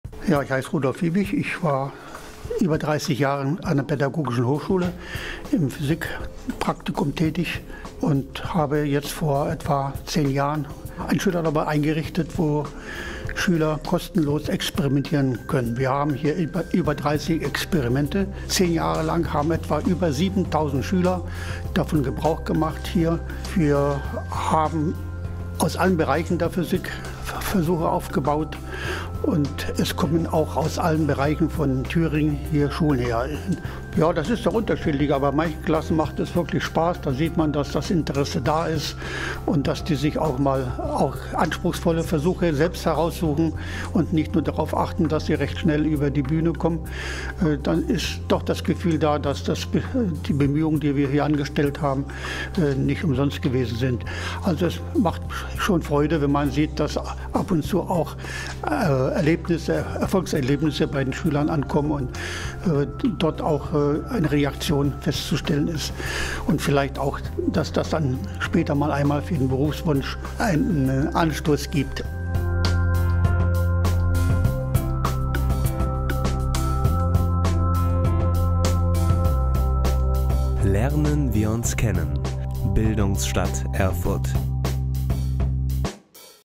Das Labor der Schüler
Bildungsstadt Erfurt, Lernen wir uns kennen!

Das Schülerlabor im Elektromuseum Erfurt
elektromuseum.de

von Andre´ Neumann